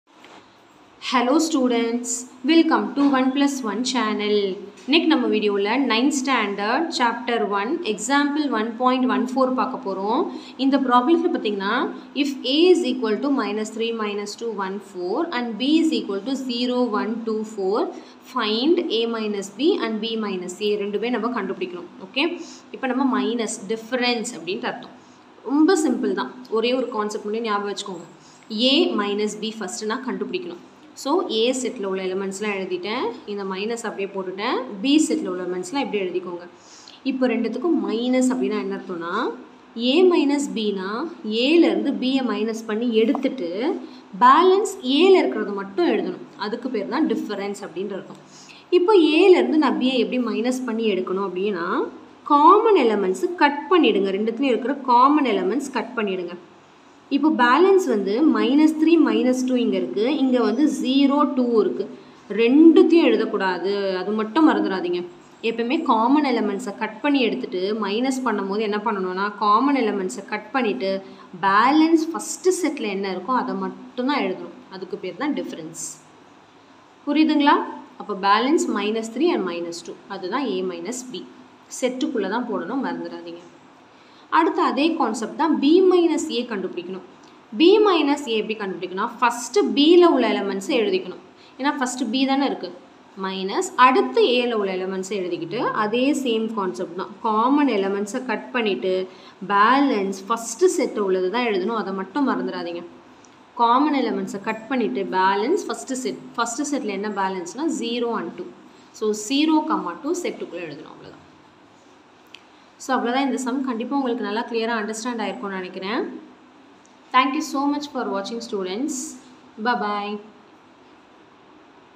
9th Maths Ch-1 Set language | Example 1.14 | Samacheer One plus One channel
This video contains for 9th standard Maths Chapter-1 Set language, Example-1.14 complete solution.

My Another Channel 👇
Sangeetha Media